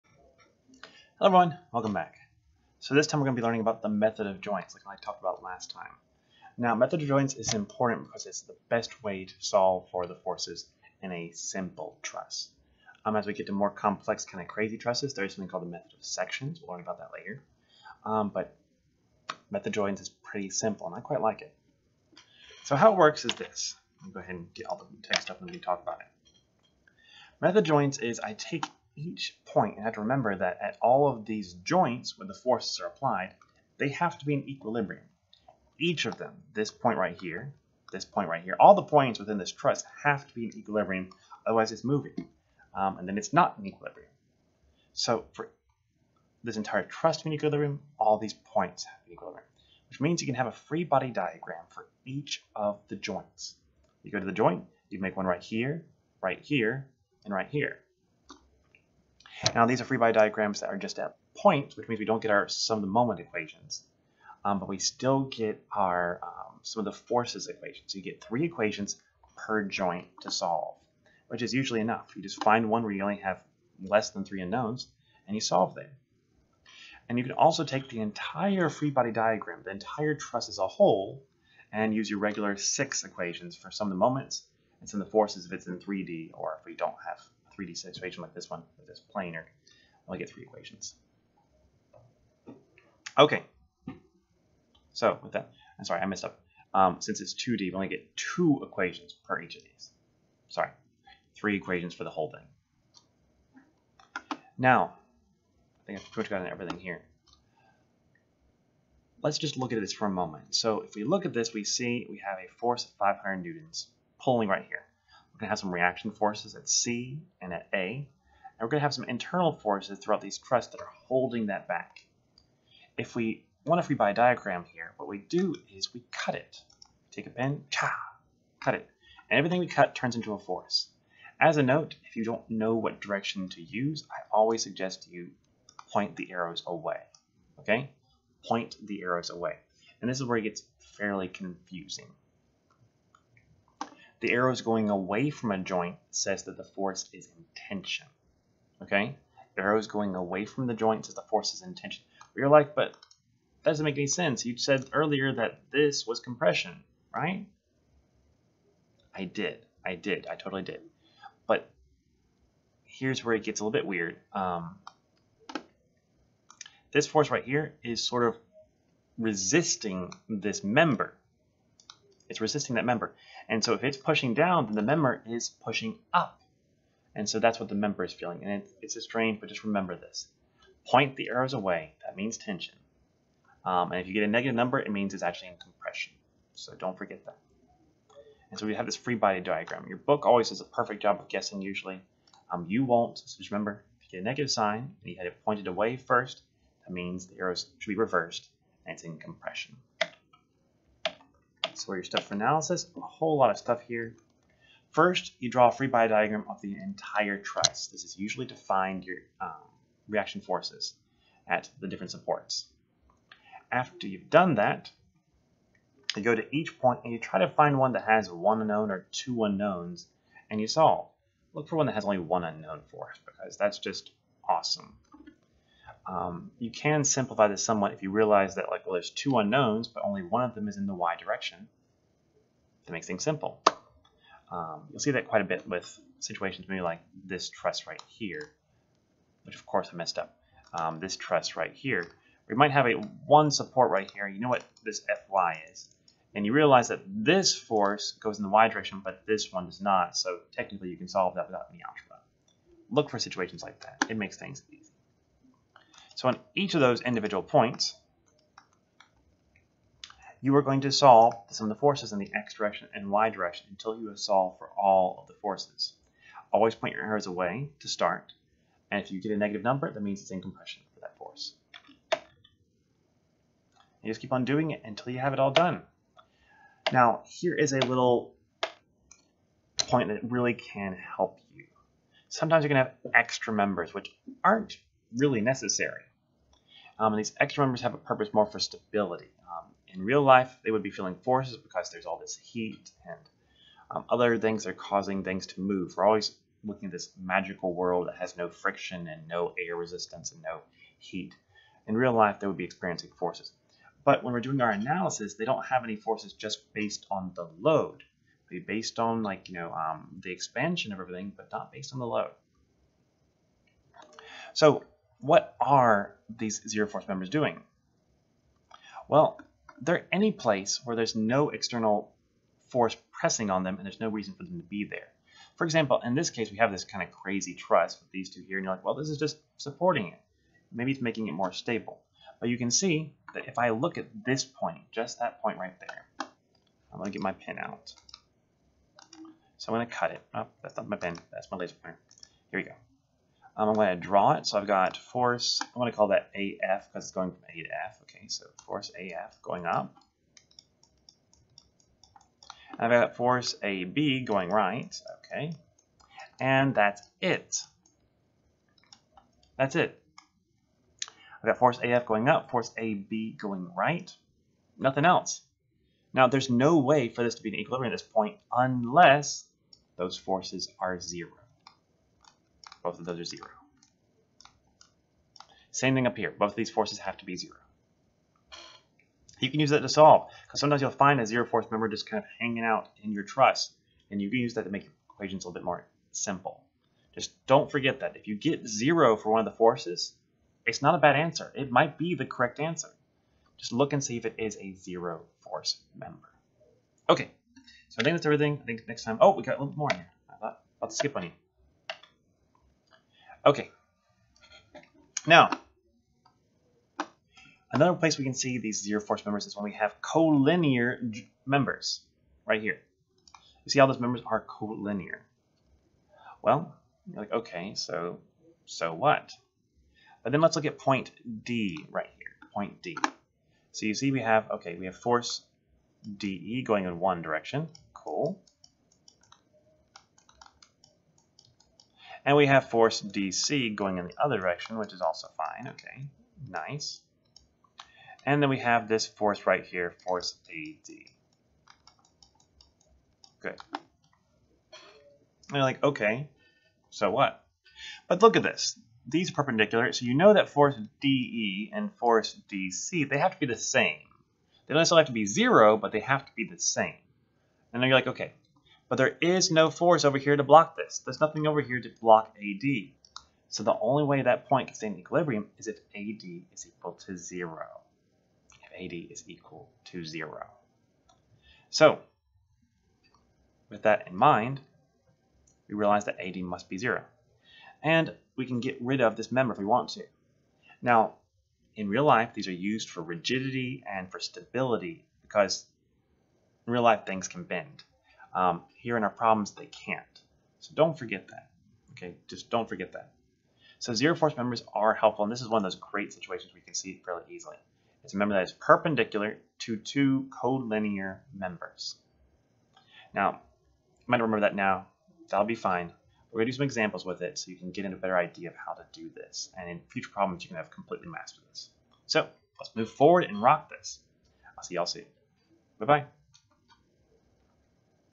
Method of Joints
Trusses and triangles are cool, but how do we figure out the forces with all of these crazy shapes?! This time, we break down the craziness and look at trusses joint by joint.